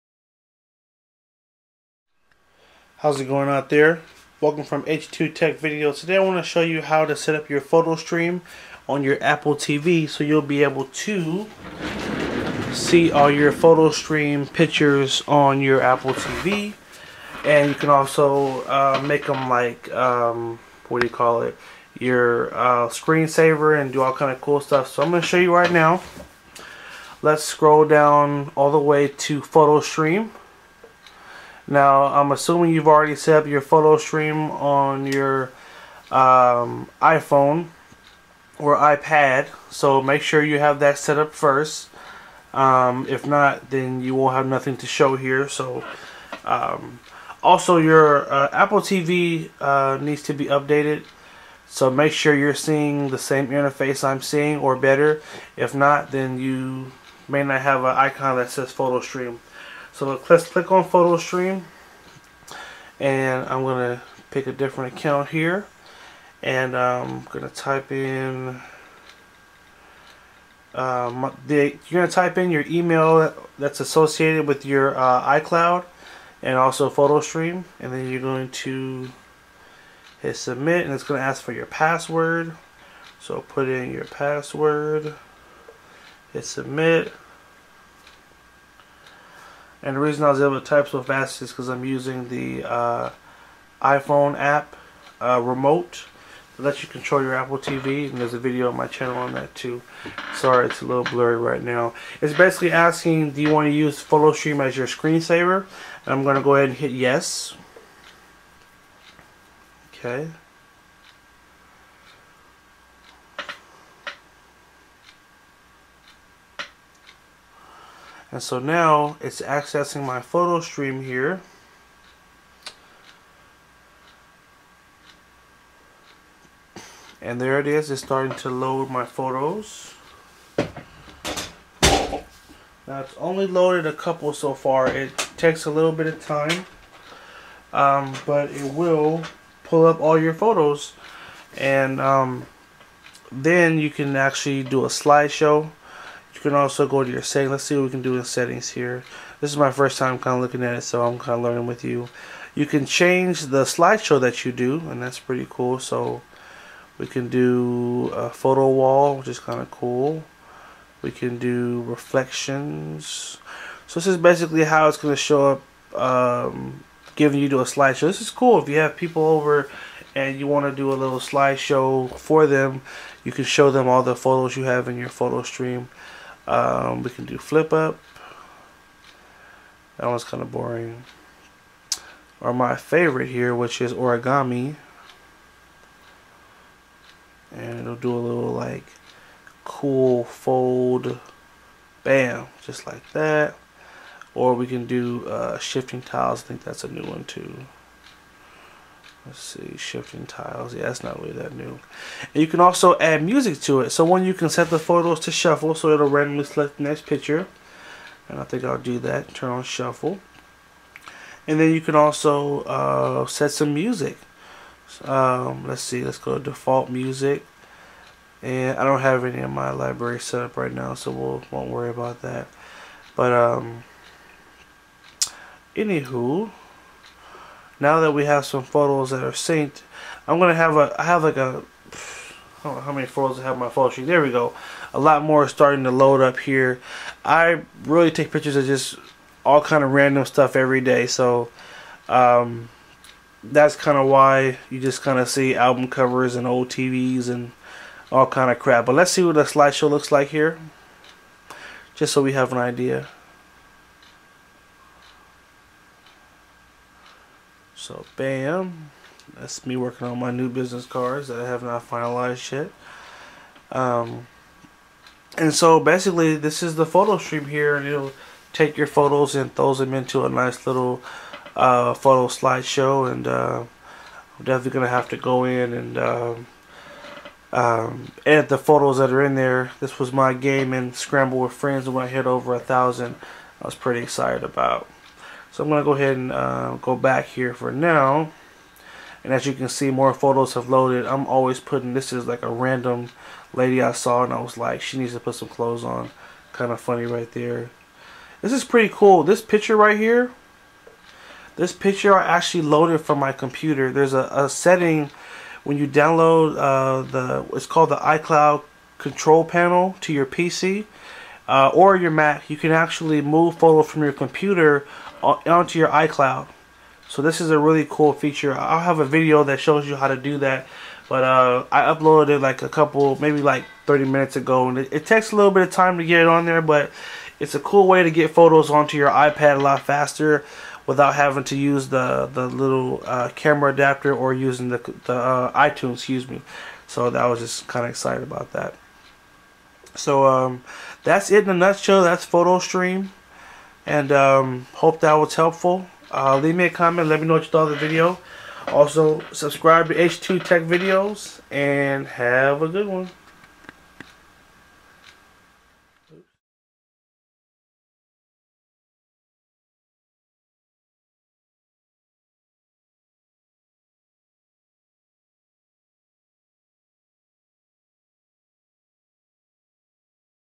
Apple TV: How to Connect Your Photostream​​​ | H2TechVideos​​​
This is a video tutorial on how to sync your photo stream on to your apple tv so you can see the pictures through Apple TV.​​​